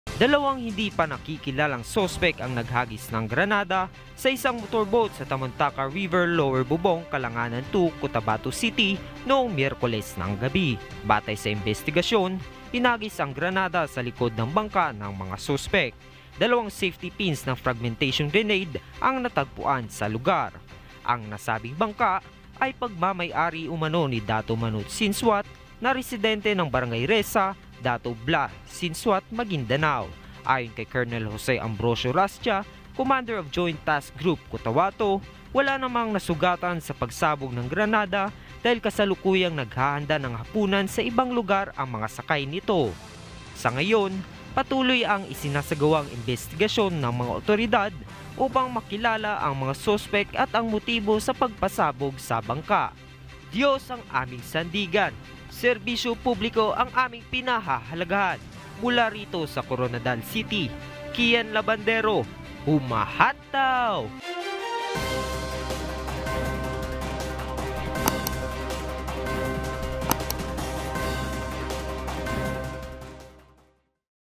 Bangka sa Tamontaka River sa Cotabato City, hinagisan ng granada
Hinagisan ng granada ng mga hindi pa nakikilalang salarin ang isang motor boat na nakadaong sa Tamontaka River sa Cotabato City.